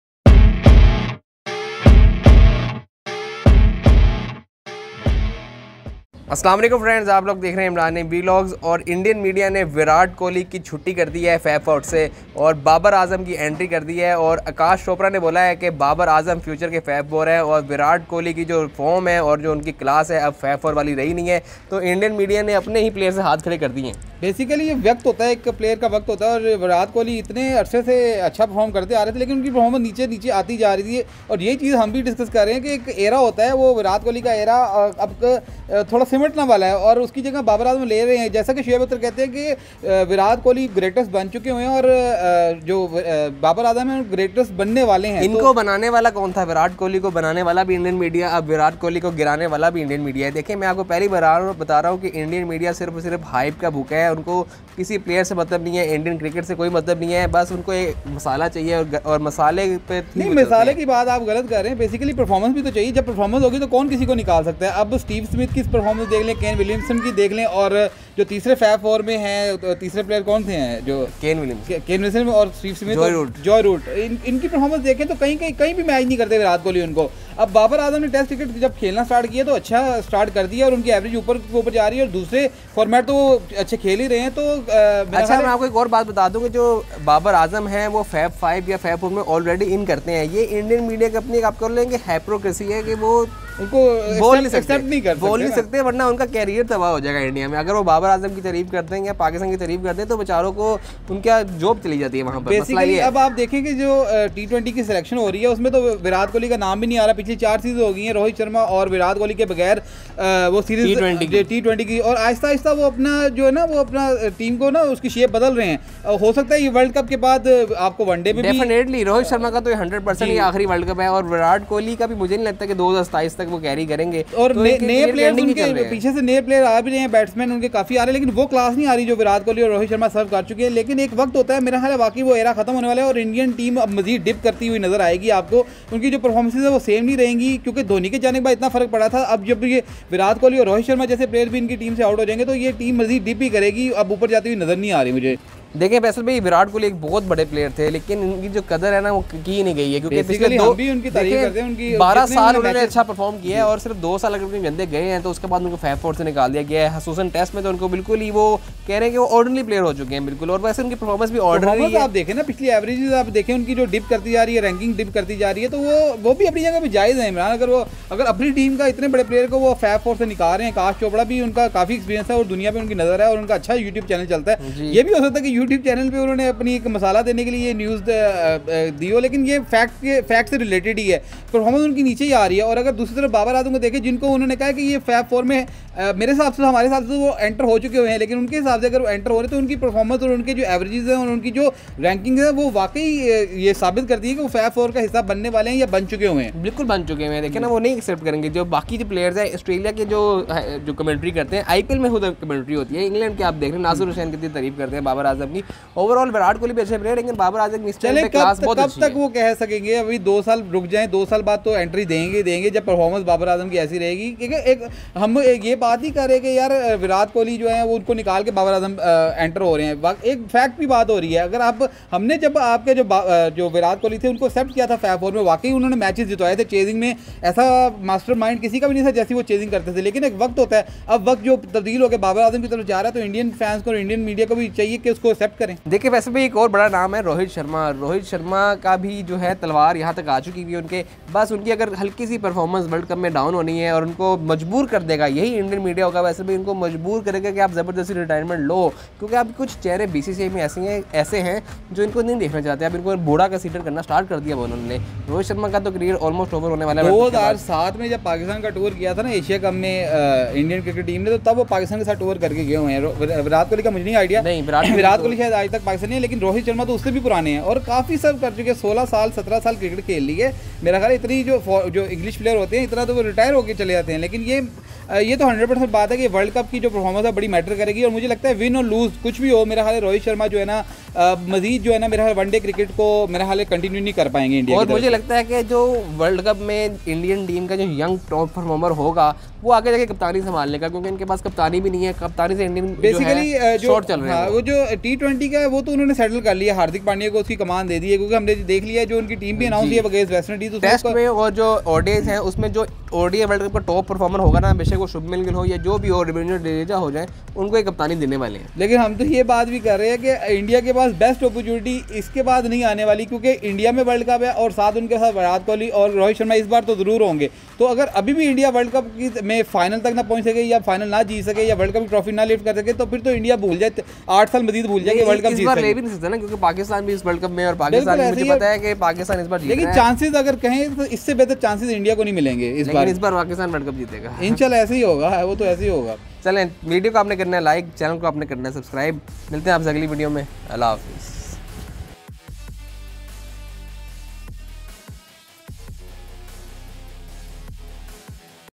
Indian Media Praise Babar Azam & Criticise Virat Kohli Performance
Indian media criticize Virat Kohli and praising Babar Azam batting Indian media says that Virat Kohli do not deserve place in fab 4 and Babar Azam should be included in Fab 4
Indian media praising Babar Azam and Virat Kohli
India v Pakistan match in ICC Cricket World Cup 2023 on 15th October